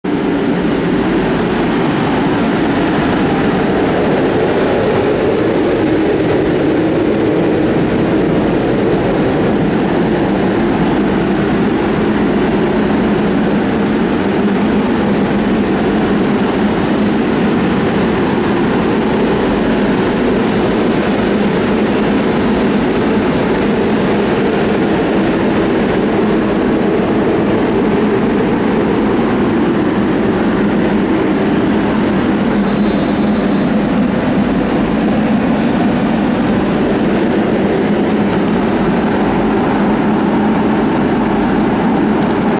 AEL-37 rayado con graffiti en la estación Alameda - 02/01/2011
- AEL-37 de Fesub rayado con graffiti en la estación Alameda.

Este automotor de Fesub que cubre el recorrido Talcahuano-Laja en la Octava Región llamado "Corto del Laja" fue llevado a Santiago para ser sometido a algunas reparaciones y mientras estaba estacionado en los andenes de la estación Alameda fue rayado con el feo  graffiti carente de talento artístico que se aprecia en el video por jovenes que buscan llamar la atención y sobresalir entre sus pares dejando sus firmas ilegibles por toda la ciudad, ya que eso les da un mayor estatus dentro de su grupo y los hace sentirse importantes y poderosos.